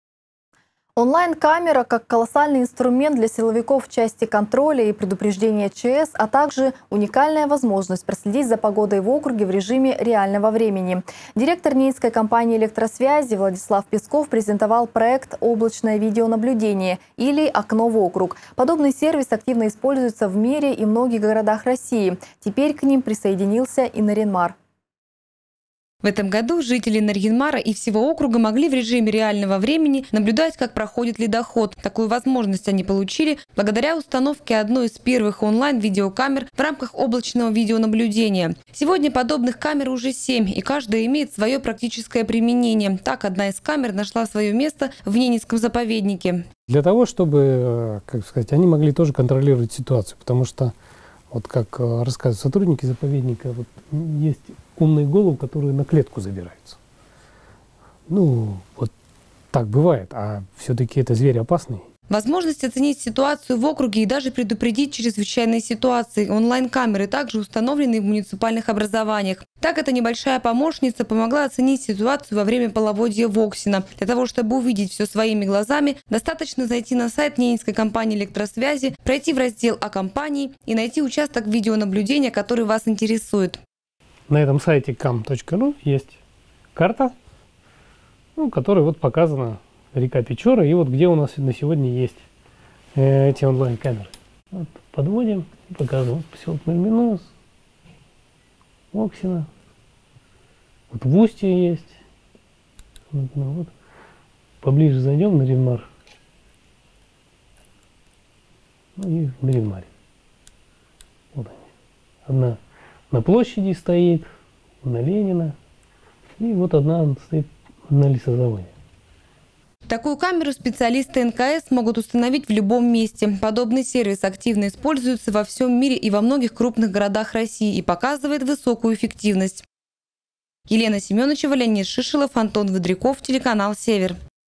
Ненецкая компания электросвязи презентовала проект «Облачное видеонаблюдение»
Онлайн камера, как колоссальный инструмент для силовиков в части контроля, предупреждения ЧС и происшествий, а также уникальная возможность проследить за погодой в округе в режиме реального времени. Директор Ненецкой компании электросвязи Владислав Песков презентовал проект «Облачное видеонаблюдение» или окно в округ. Подобный сервис активно используется в мире и многих городах России. Теперь к ним присоединился и Нарьян-Мар.